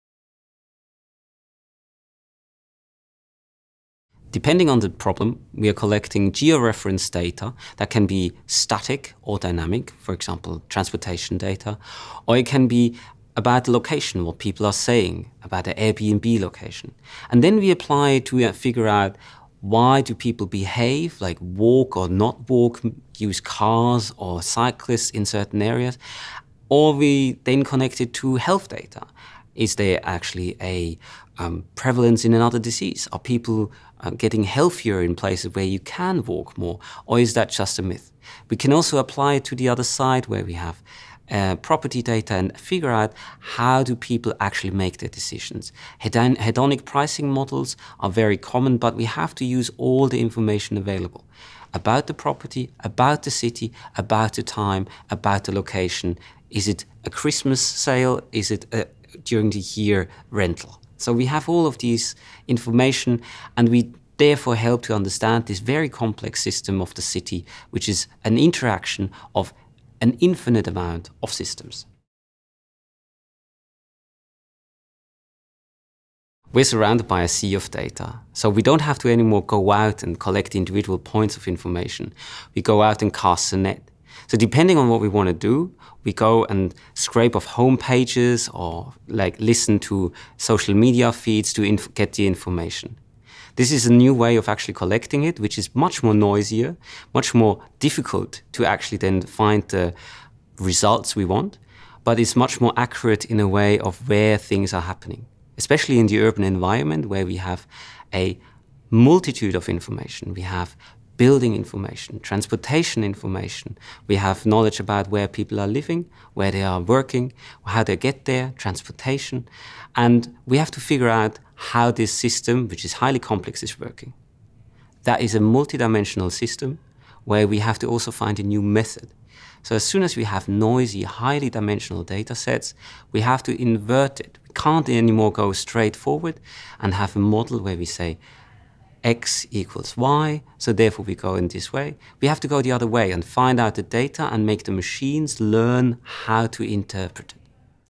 Data and Research - how to get it and make sense of it.
General thoughts on Data:
What kind of data?
How to collect it?

We are surrounded by a sea of data.